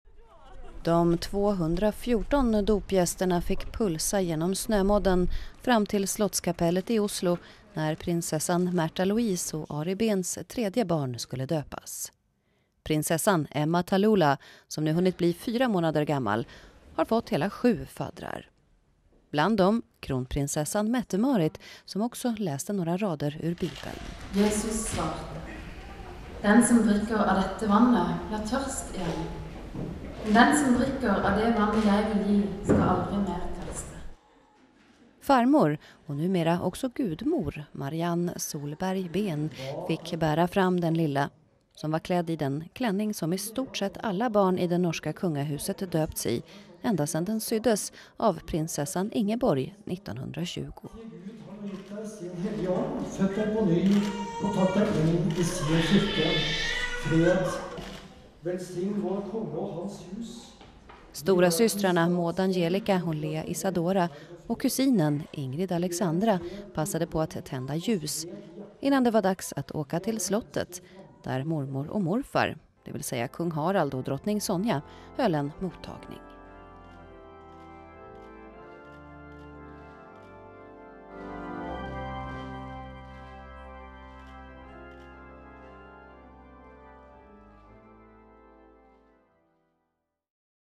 Prinsessan Märtha Louises & Ari Behns dotter döps
Prinsessan Märtha Louise och Ari Behns tredje dotter Emma Tallulah döptes i Slottskapellet i Oslo på tisdagen. Norges yngsta prinsessa har begåvats med sju faddrar, bland dem kronprinsessan Mette-Marit.